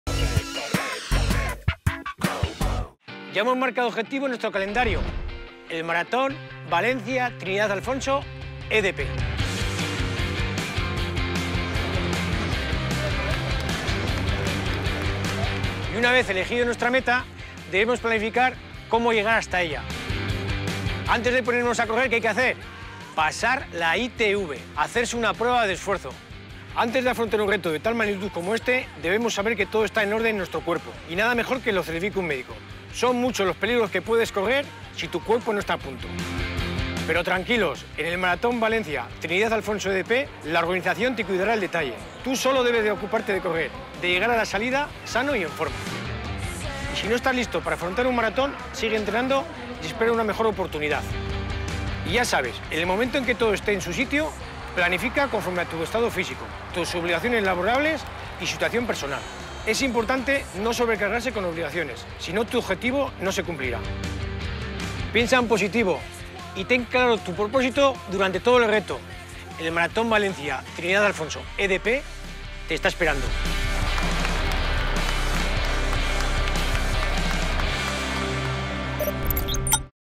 El Maratón de Valencia te está esperando | CORREDOR
Martín Fiz te da los mejores consejos para afrontar la preparación del próximo Maratón Valencia Trinidad Alfonso EDP.